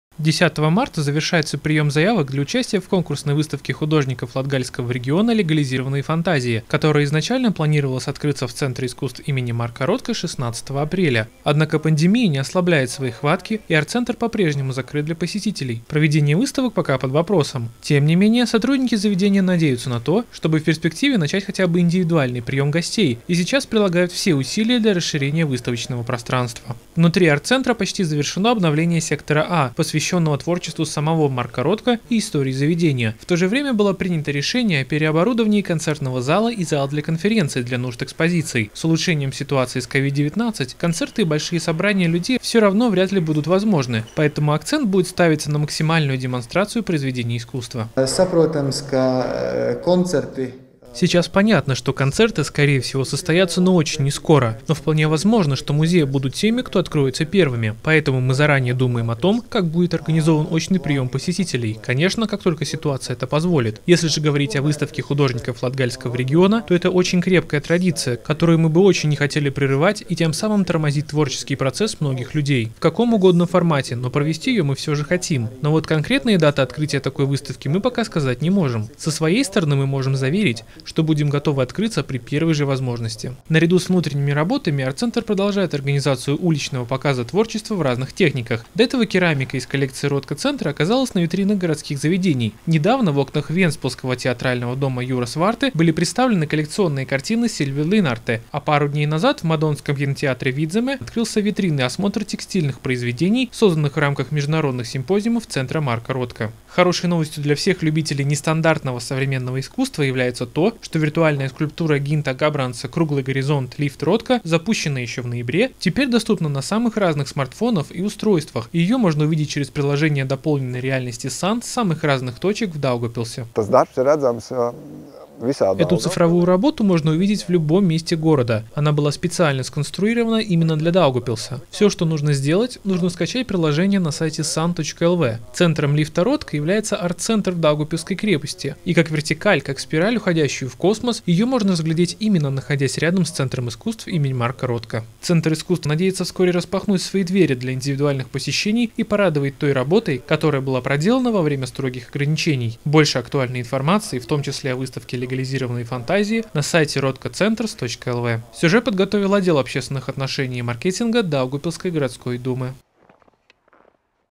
Арт-центр Марка Ротко нацелен на очный прием посетителей
10 марта завершается прием заявок для участия в конкурсной выставке художников латгальского региона «Легализованные фантазии», которая изначально планировалась открыться в Центре искусств им. Марка Ротко 16 апреля. Однако пандемия не ослабляет своей хватки и арт-центр по-прежнему закрыт для посетителей - проведение выставок пока под вопросом. Тем не менее, сотрудники заведения надеются на то, чтобы в перспективе начать хотя-бы индивидуальный прием гостей и сейчас прилагают все усилия для расширения выставочного пространства.